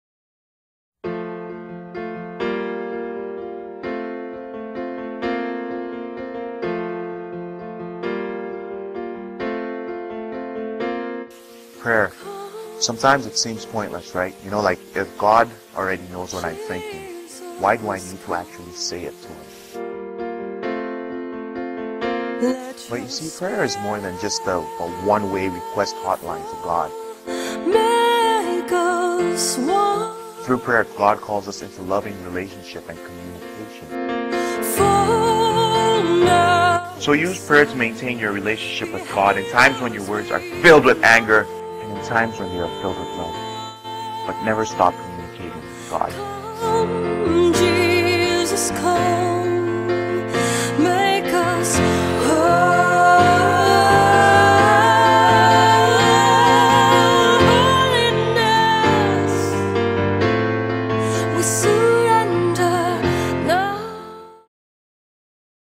Keeping the Lines Open
Prayer sometimes seems pointless, but it's not. Prayer is meaningful in many ways.  How and why should we keep the lines open with God?

Do not be anxious about anything, but in every situation, by prayer and petition, with thanksgiving, present your requests to God.
Philippians 4:6